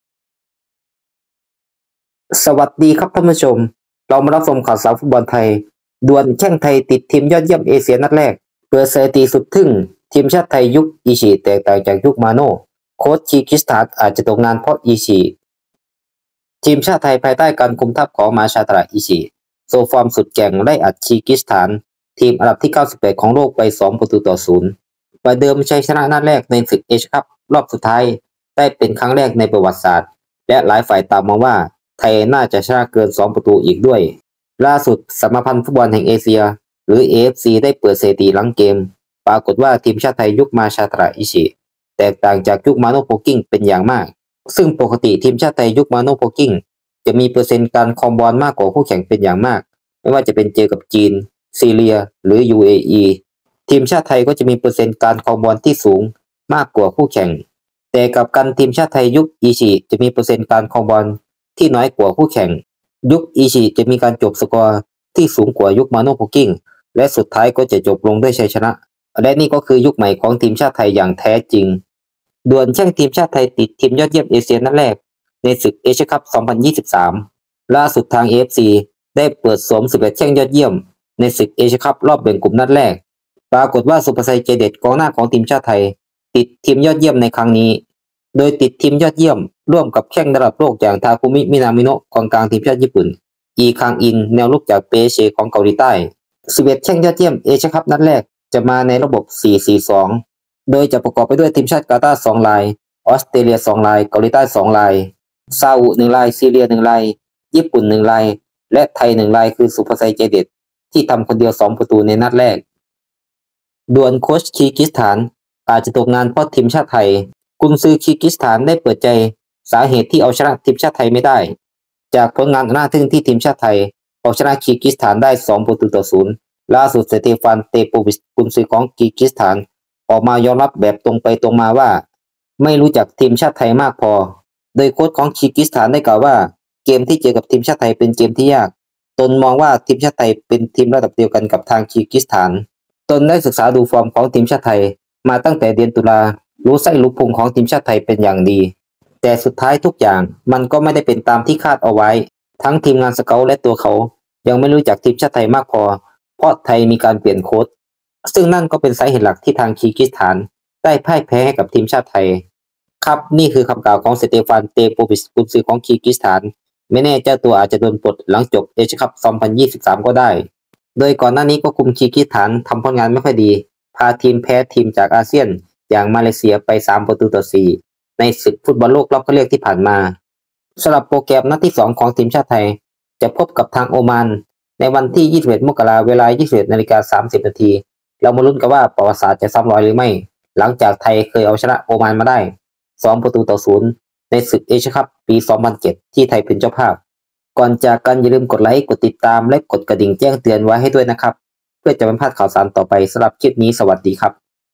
ทึ่ง! 2อย่างที่ทีมชาติไทยยุค อิชิอิ ต่างจากยุค มาโน่ ด่วน!แข้งไทยติด ยอดเยี่ยมเอเชีย
ข่าวสารฟุตบอลไทย เอเชียนคัพ2023 มาชาทาดะ อิชิอิ